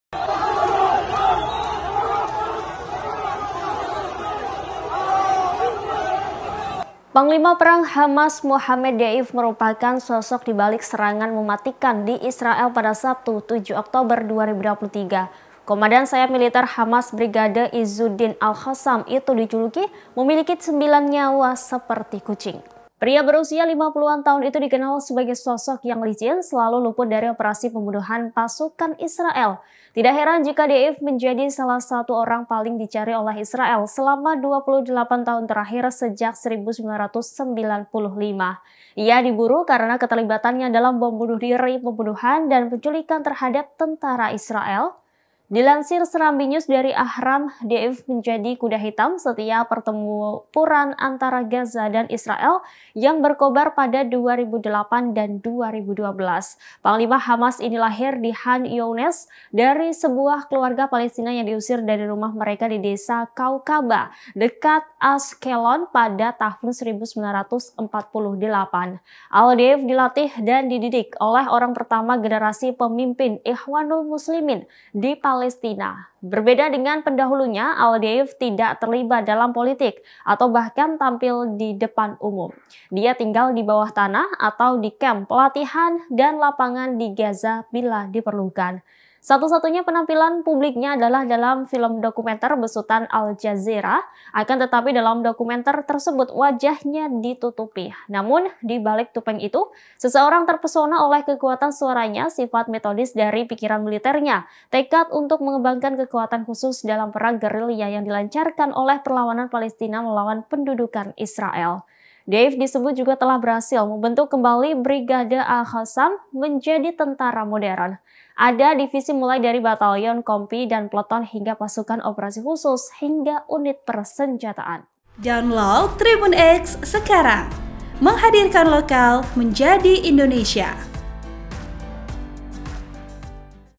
SOSOK Panglima Perang Hamas yang Ditakuti Israel Bernama Mohammad Deif, Dijuluki Kucing Bernyawa 9
TRIBUN-VIDEO.COM - Panglima perang Hamas Mohammed Deif merupakan sosok di balik serangan mematikan di Israel pada Sabtu (7/10/2023).

Komandan sayap militer Hamas Brigade Izzuddin Al Qassam itu dijuluki punya 9 nyawa seperti kucing.

Pria berusia 50-an tahun itu dikenal sebagai sosok yang licin, selalu luput dari operasi pembunuhan pasukan Israel.

Tak heran jika Deif menjadi salah satu orang paling dicari oleh Israel selama 28 tahun terakhir sejak 1995.

Ia diburu karena keterlibatannya dalam bom bunuh diri pembunuhan dan penculikan terhadap tentara Israel.

Dilansir Serambinews dari ahram, Deif menjadi kuda hitam setiap pertempuran antara Gaza dan Israel yang berkobar pada 2008 dan 2012.

Panglima Hamas ini lahir di Khan Younes dari sebuah keluarga Palestina yang diusir dari rumah mereka di desa Kawkaba, dekat Ashkelon, pada tahun 1948.

Al deif dilatih dan dididik oleh orang pertama generasi pemimpin Ikhwanul Muslimin di Palestina.

Berbeda dengan pendahulunya, Al-Deif tidak terlibat dalam politik, atau bahkan tampil di depan umum.

Dia tinggal di bawah tanah, atau di kamp pelatihan dan lapangan di Gaza, bila diperlukan.

Satu-satunya penampilan publiknya adalah dalam film dokumenter besutan Al-Jazeera.

Akan tetapi dalam dokumenter wajahnya ditutupi.

Namun di balik topeng itu, seseorang terpesona oleh kekuatan suaranya, sifat metodis dari pikiran militernya, tekad untuk mengembangkan kekuatan khusus dalam perang gerilya yang dilancarkan oleh perlawanan Palestina melawan pendudukan Israel.

Deif disebut juga telah berhasil membentuk kembali Brigade Al-Qassam menjadi tentara modern.

Ada divisi mulai dari batalyon, kompi dan peleton hingga pasukan operasi khusus hingga unit persenjataan.

Artikel ini telah tayang di SerambiNews.com dengan judul VIDEO - Sosok Mohammed Deif, Komandan Militer Hamas Tinggal di Bawah Tanah, Pimpin Badai Al Aqsa,